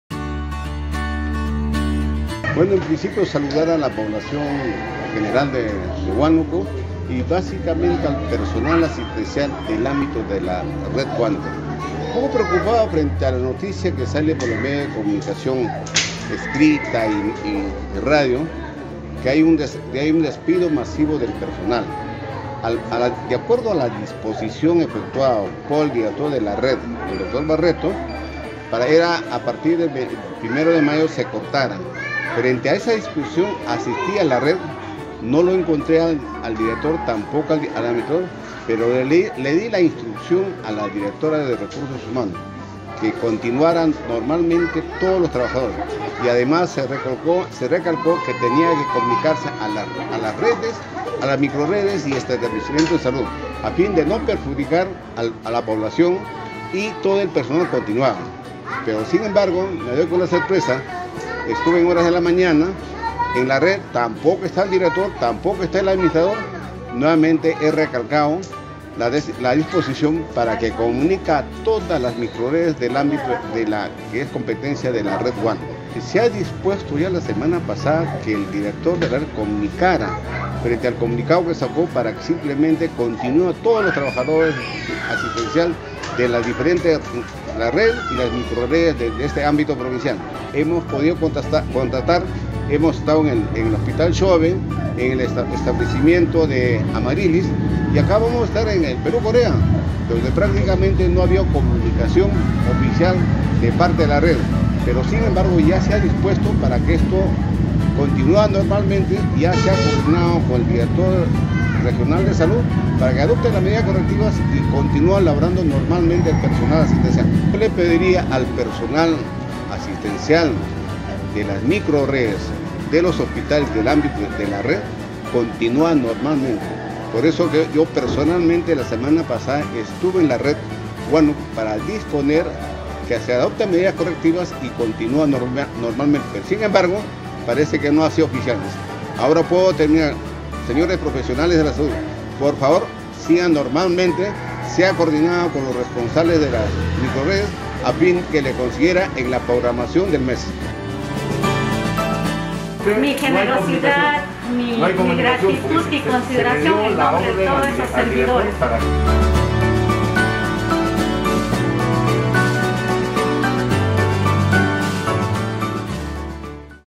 🧑‍⚕️ Salud II GERENTE DESARROLLO SOCIAL RATIFICA QUE PERSONAL ASISTENCIAL CONTINUARÁN TRABAJANDO
Gustavo Alvarado Coz, titular de la Gerencia de Desarrollo Social del Gobierno Regional Huánuco (Gorehco) aseveró que el personal asistencial de las microrredes y establecimientos de salud de la jurisdicción de la Red Salud Huánuco, que laboraban bajo el régimen Contrato Administrativo de Servicios (CAS) seguirán trabajando.
"Un gobierno de transparencia e integración"